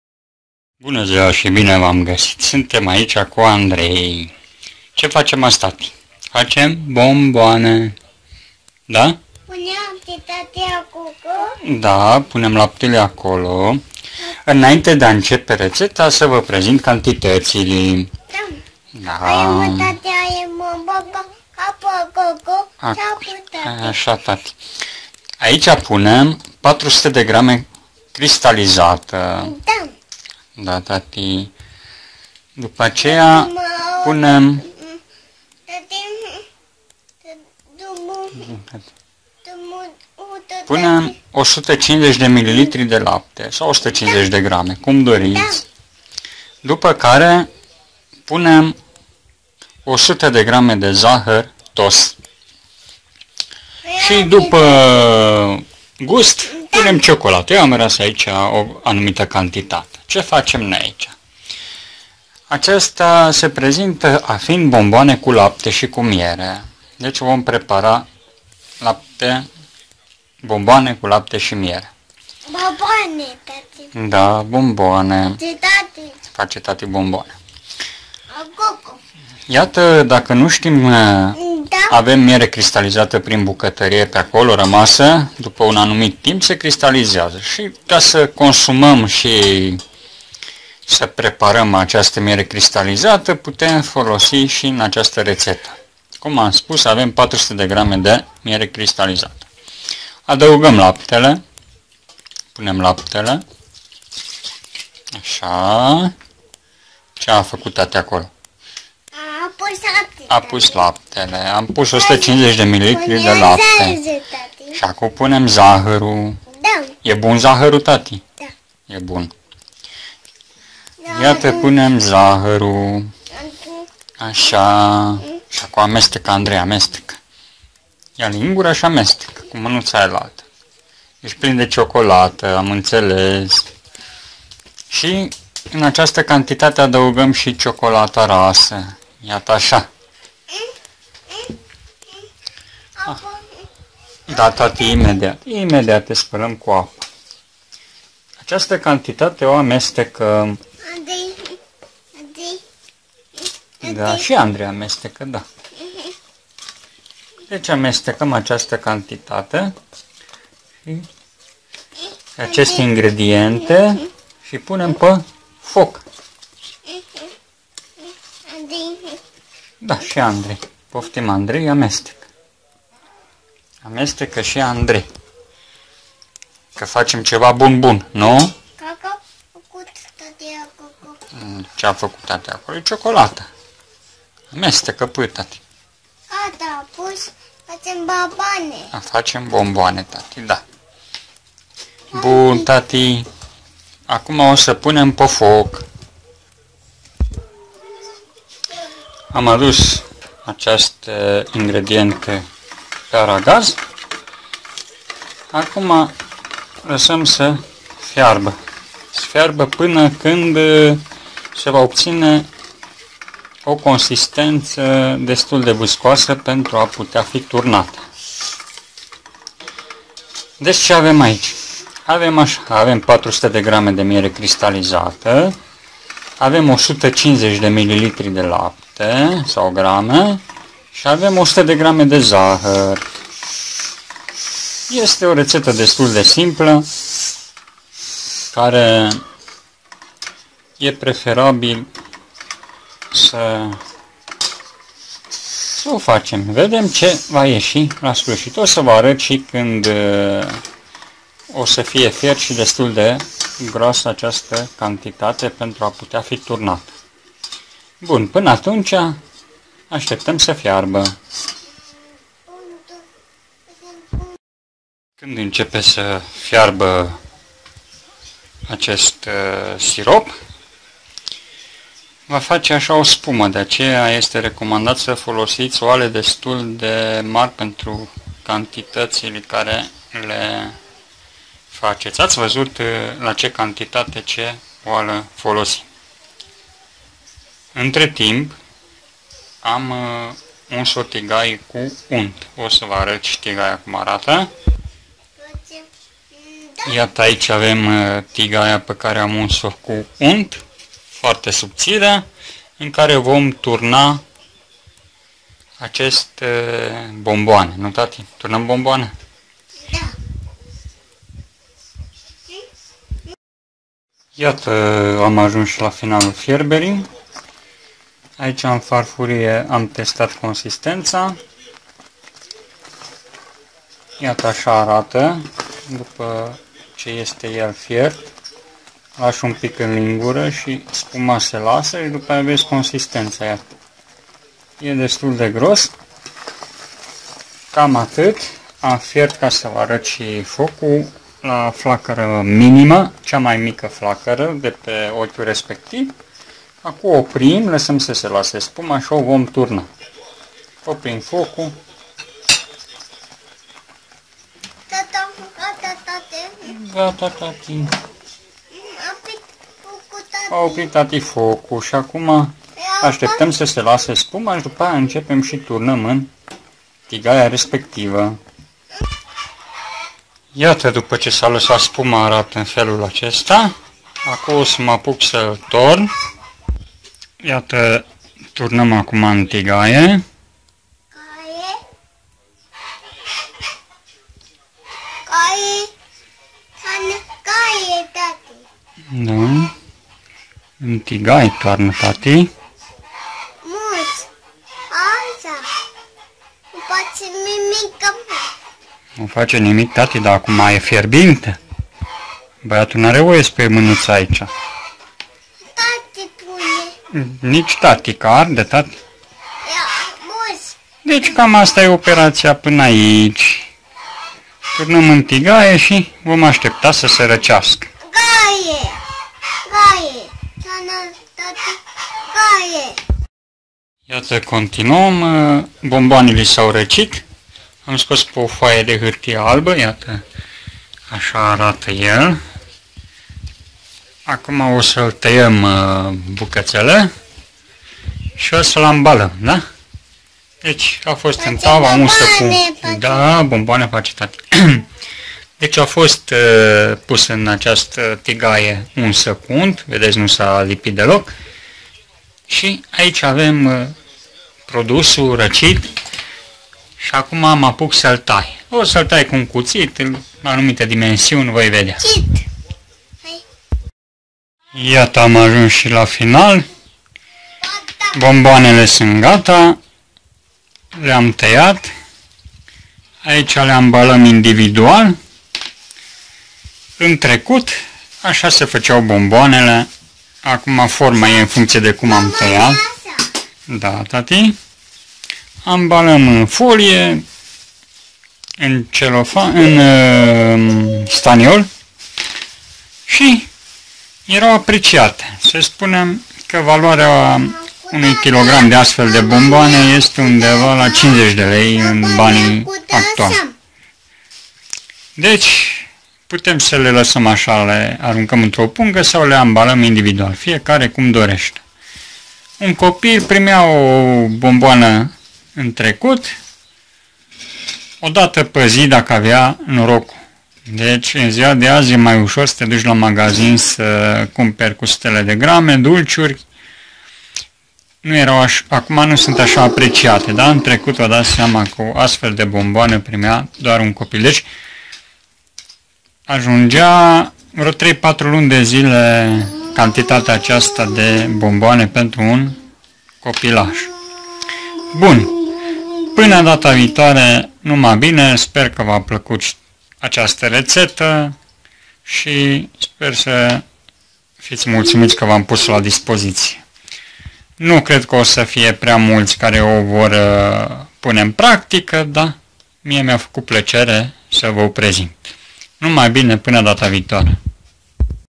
Mierea In Bucatarie ,preparare si reteta, bomboane cu lapte si miere
Iata dupa claltite un alt preparat care ami aducea bucurie in copilarie sunt bomboanele , intradevar in trecut copii primeau foarte putin acest produs dulce dar era de calitate , in zilele noastre in alergatura comerciantilor dupa venit sa ajuns sa cosumam doar zahar , iata aici o reteta in care mierea este la loc de frunte la acest preparat . Daca doar 10 % din bomboanele din Romania ar contine miere nu ar mai fi problema cu unde asi vand apicultorii mierea , dar din pacate nu este asa , iar copii primesc dulciuri mult sub calitate .